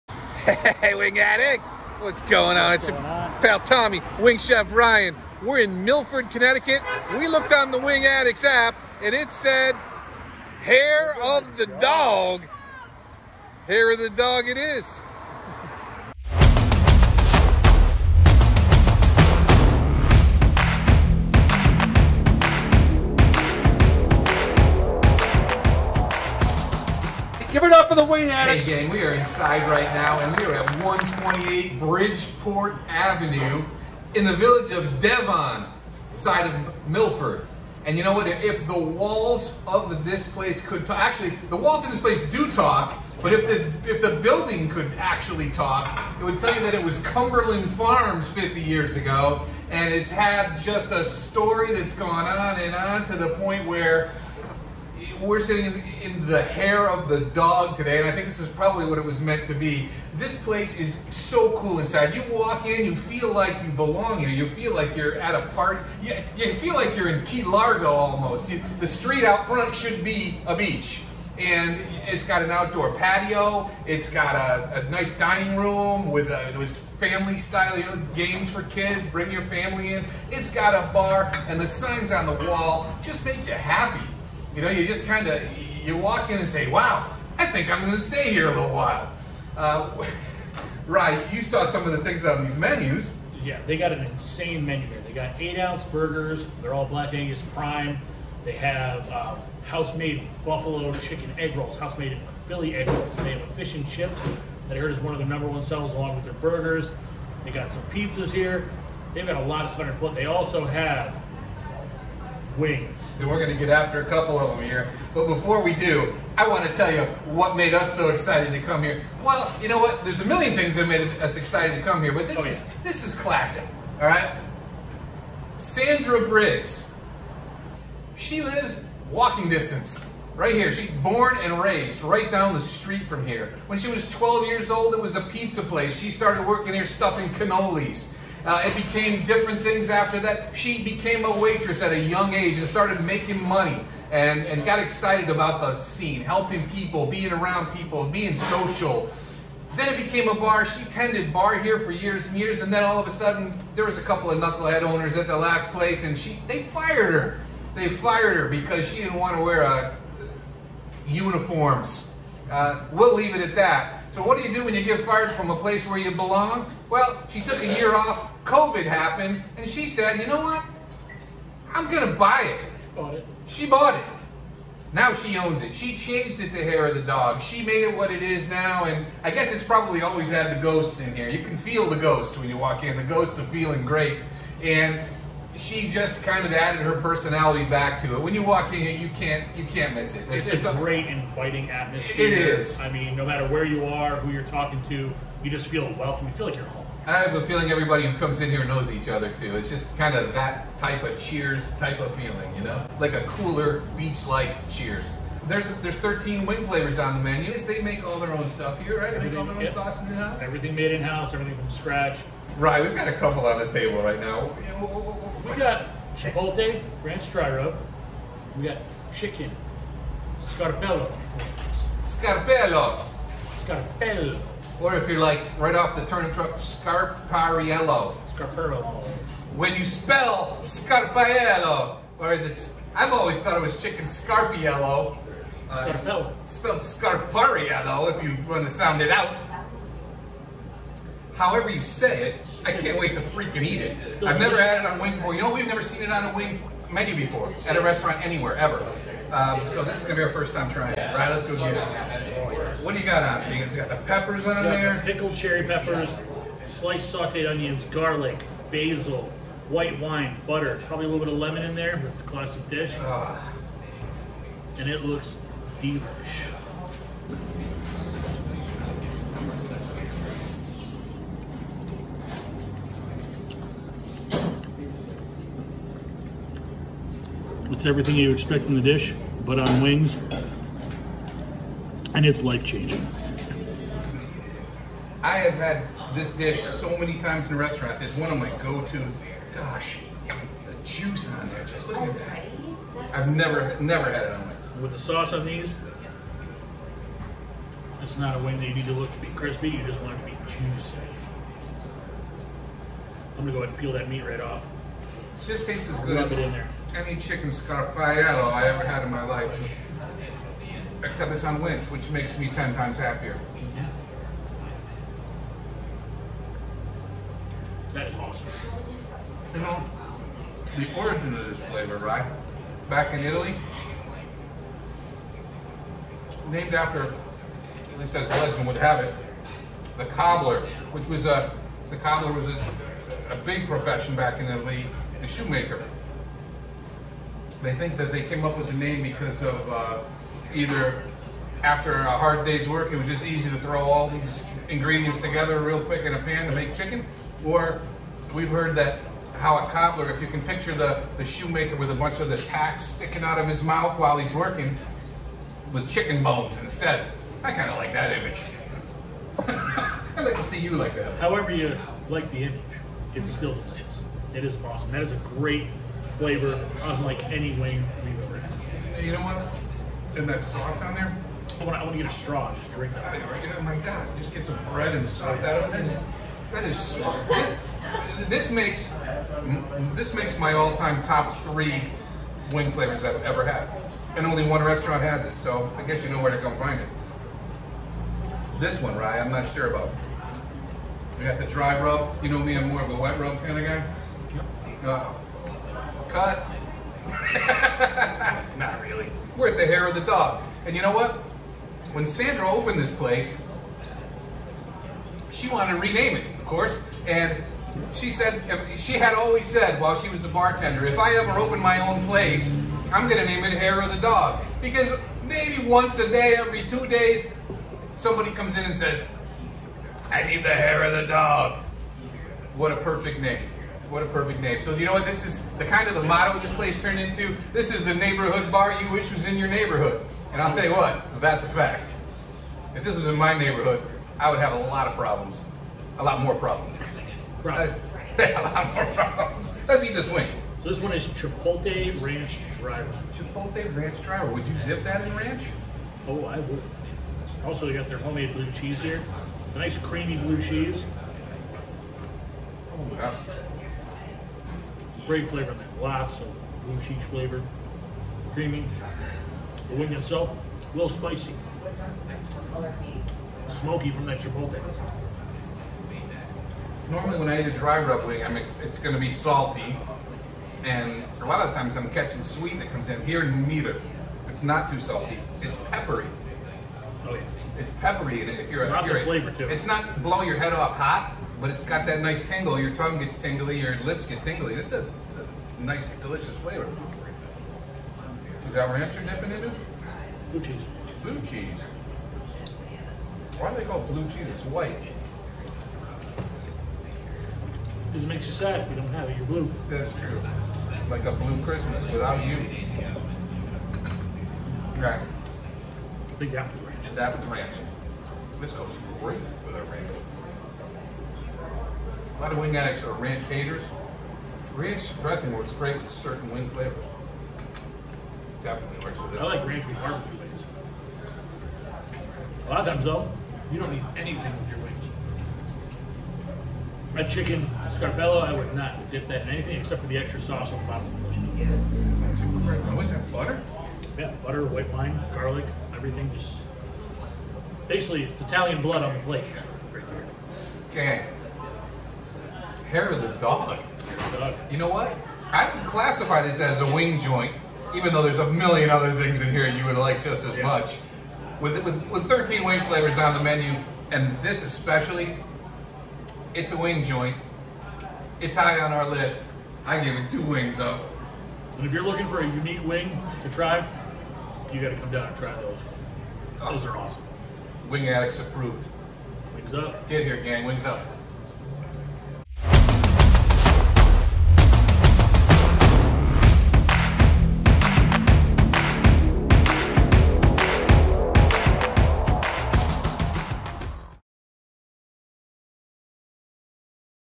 WINGADDICTS (S10: E18) Hair of the Dog in Milford (Devon) CT
Sometimes you walk into a place and its everything you want. That just happened at Hair of the Dog. The story is amazing, the vibe is amazing, the people are amazing and the food is - well - amazing. Thirteen wing flavors and wait until you see the ones WE got!
NEW Wingaddicts flavors # 639 and 640!